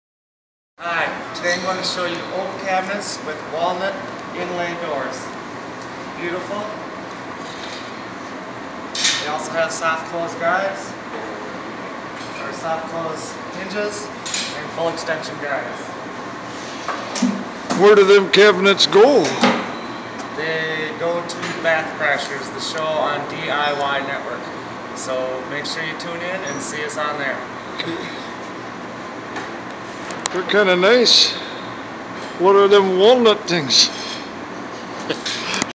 Ben with DIY Network's TV Show "Bath Crashers" cabinets
Here are the two small vanity cabinets that the DIY Network asked us to build to use in a bathroom remodel show they produce called  "Bath Crashers".